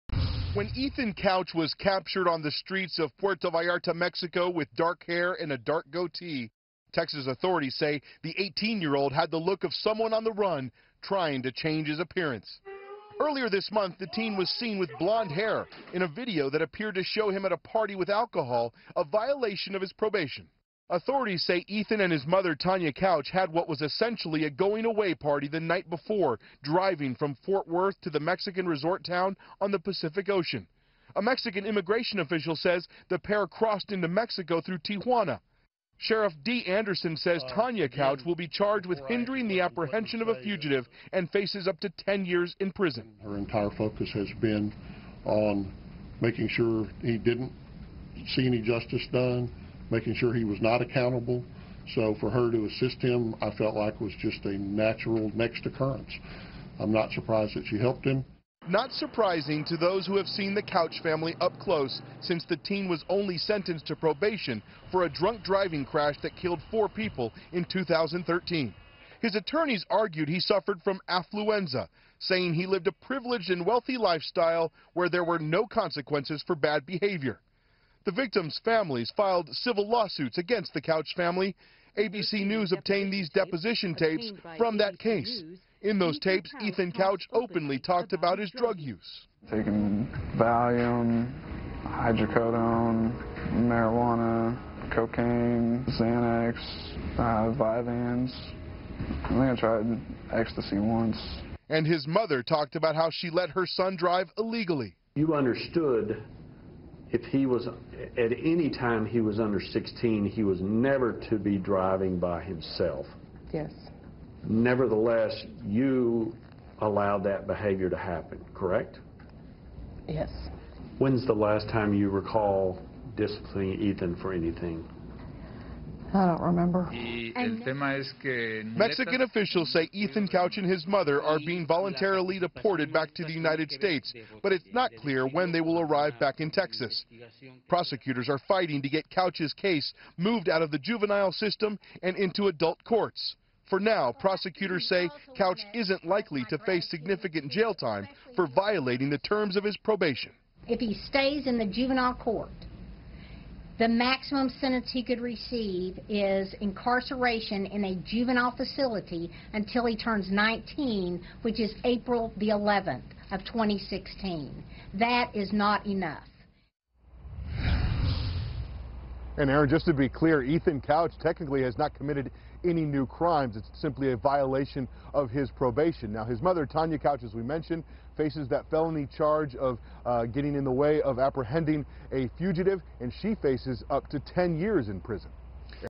'Affluenza' teen Ethan Couch detained in Mexico
Ethan Couch, the teen who used an "affluenza" defense to win a light sentence after killing 4 people in a drunk driving crash, was detained in Mexico for violating his probation. Ed Lavandera reports.